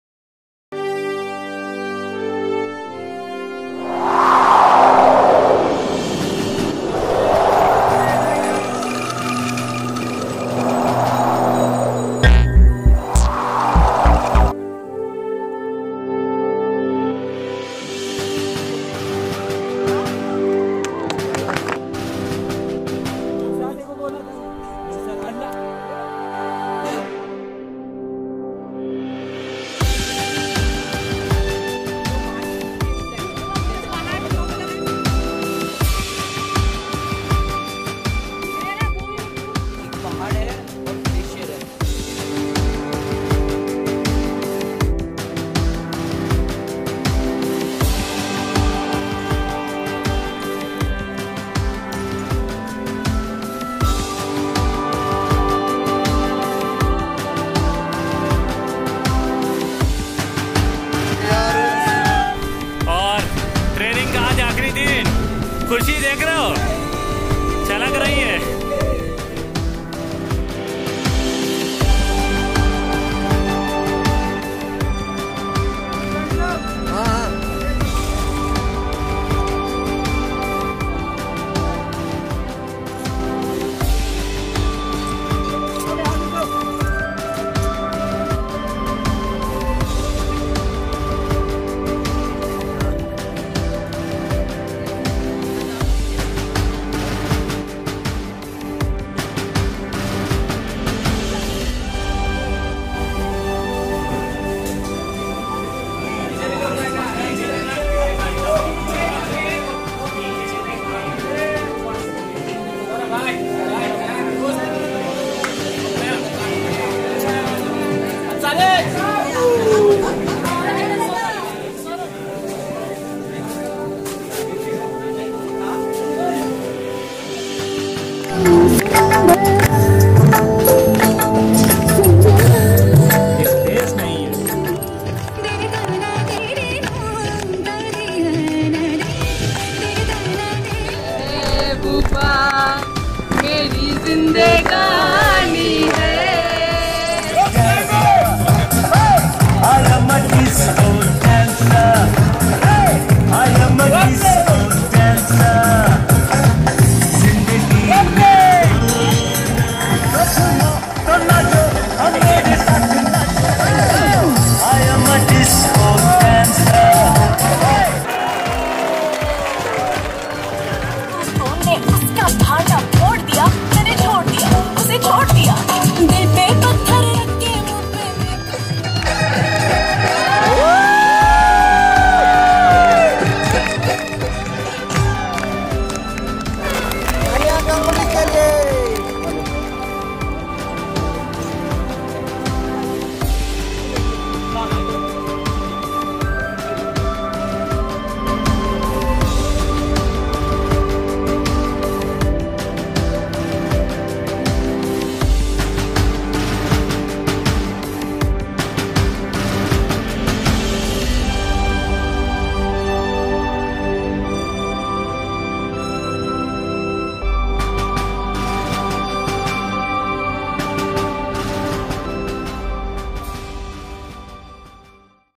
Basic mountaineering course part 3
Mountaineering course at Sonamarg
One of beautiful place of Jammu and Kashmir

JIM&WS is providing a very good and effective
  course of Mountaineering

Mountaineering. Mountaineering
course. Ladakh video. Ladakh. Basic mountaineering Course. Adventure course.